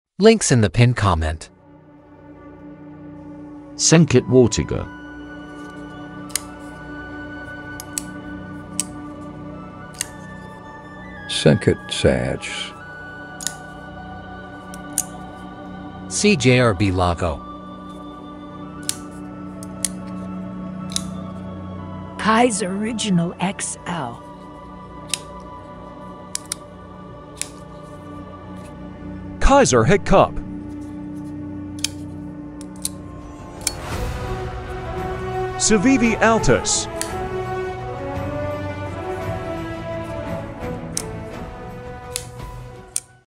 6 Awesome Button Lock Knives
6 Great High Value Button Lock Knives

❎Sencut - Watauga

❎Sencut- Sachse

❎CJRB - Lago

❎Kizer- Original XL

❎Kizer - Hic Cup

❎Civivi - Altus